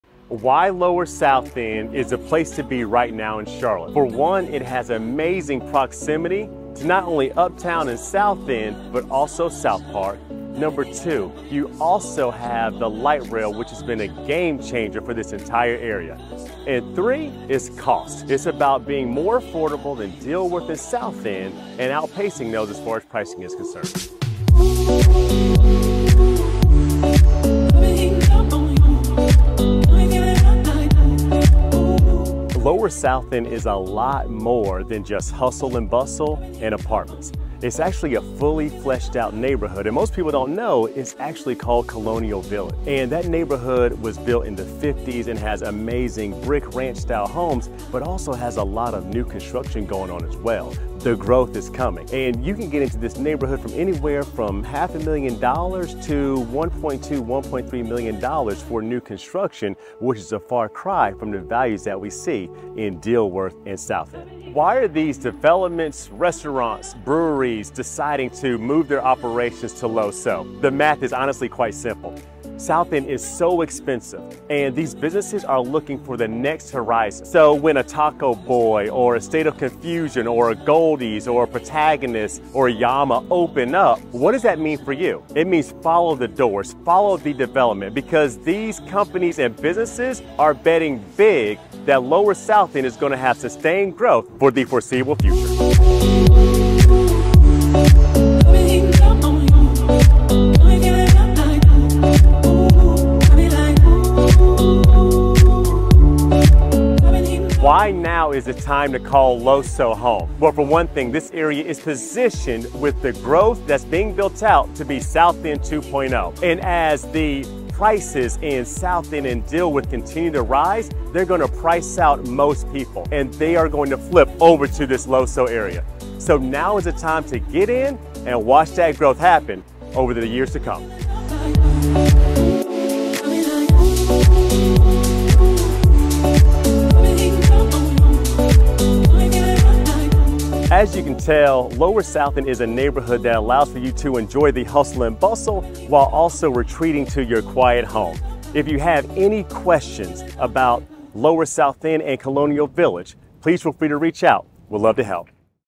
Charlotte Neighborhood Tour: Lower SouthEnd
Lower South End in Charlotte is the place to be for several reasons. In terms of convenience, Lower South End is well-connected to other parts of Charlotte. It is situated just south of Uptown, making it easily accessible for commuters. The light rail system provides convenient transportation options for residents and visitors alike.

Secondly, Lower South End boasts excellent dining and entertainment options. It is often referred to as the “brewery district” due to its numerous craft breweries and taprooms. Food enthusiasts can explore a wide range of culinary delights, from trendy eateries to international cuisines.

Lastly, it offers a unique blend of history and modernity. The neighborhood is known for its revitalized industrial buildings, which have been transformed into trendy breweries, restaurants, and art galleries. This mix of old and new creates a vibrant and dynamic atmosphere.

Overall, Lower South End’s unique blend of history, creativity, and entertainment make it a desirable place to live, work, and explore in Charlotte.